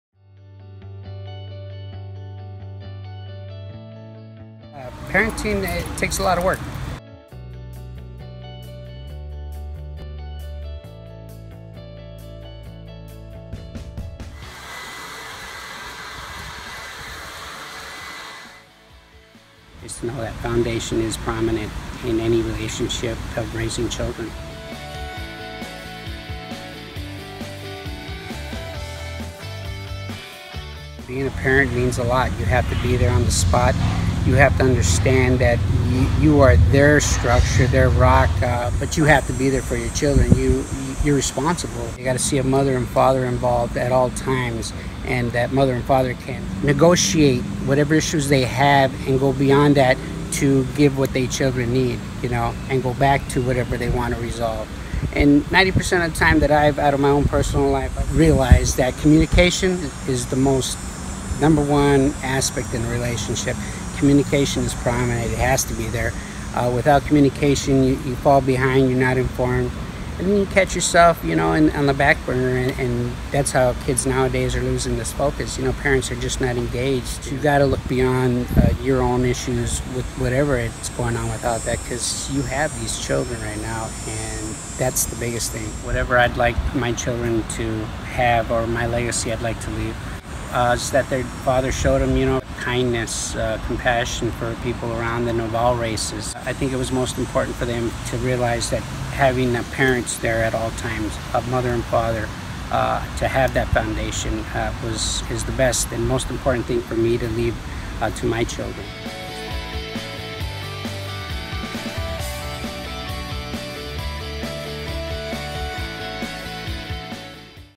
Jose Verduzco - MFI Summit
MFI would like to thank Jose Verduzco for taking time out of his schedule to talk about his perspective on fatherhood, family, and accountability.